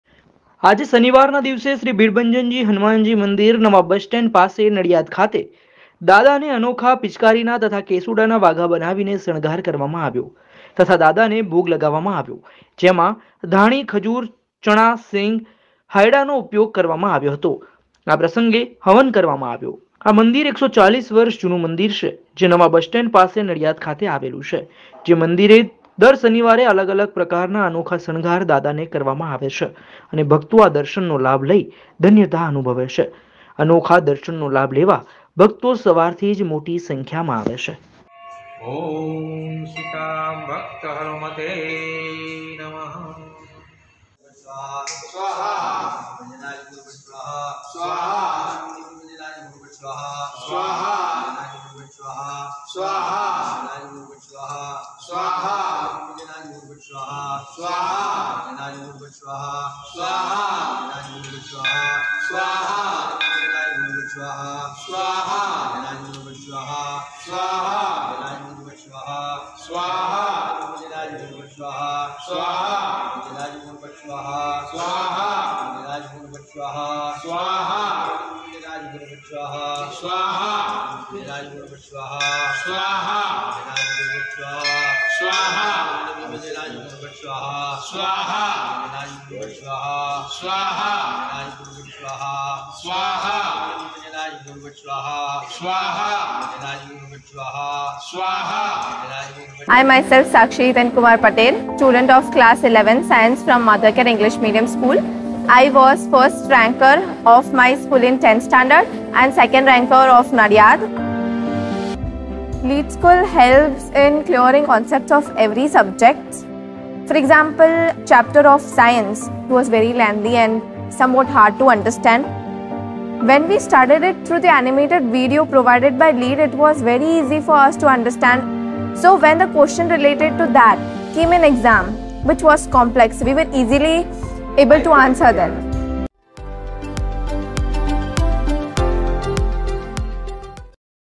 NADIAD : શ્રી ભીડભંજન હનુમાનજી દાદાને પિચકારીના તથા કેસુડાના વાઘા બનાવીને શણગાર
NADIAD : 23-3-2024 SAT
આજે શનિવારના દિવસે  શ્રી ભીડભંજન હનુમાનજી મંદિર નવા બસ સ્ટેન્ડ પાસે નડિયાદ ખાતે દાદાને અનોખા પિચકારીના તથા કેસુડાના વાઘા બનાવી ને શણગાર કરવામાં આવ્યા તથા દાદાને ભોગ લગાવવામાં આવ્યો. જેમાં દાણી ખજૂર ચણા સિંગ આઈડા નો ઉપયોગ કરવામાં આવ્યો.
આ પ્રસંગે હવન કરવામાં આવ્યો.
આ મંદિર  140 વર્ષ જૂનું મંદિર છે જે નવા બસ સ્ટેન્ડ પાસે નડિયાદ ખાતે આવેલું છે.
જે મંદિરે દર શનિવારે અલગ અલગ પ્રકારના અનોખા શણગાર દાદાને કરવામાં આવે છે અને ભક્તો આ દર્શનનો લાભ લઈ ધન્યતા અનુભવે છે.
અનોખા દર્શન નો લાભ લેવા ભક્તો સવારથી મોટી સંખ્યામાં આવે છે..